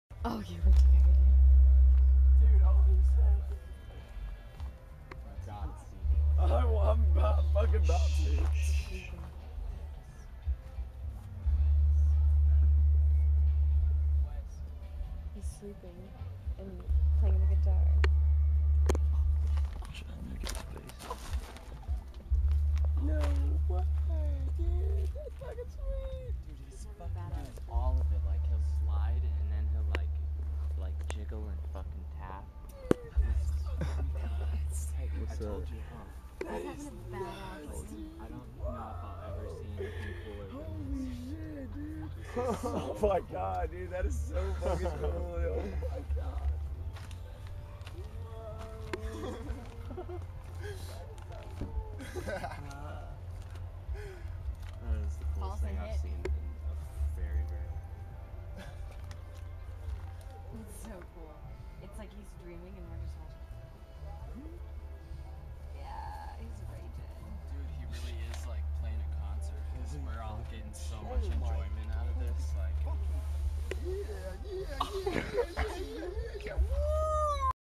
Wakarusa 2011 Kid Lucid Dreaming While Playing the Guitar in His Dreams!
Kid passed out during the peak of the night at Wakarusa Music Festival in Ozark, Arkansas and this is what crazy shit was goin on in his head. Shredding it! Woooooooooooooooooooooo!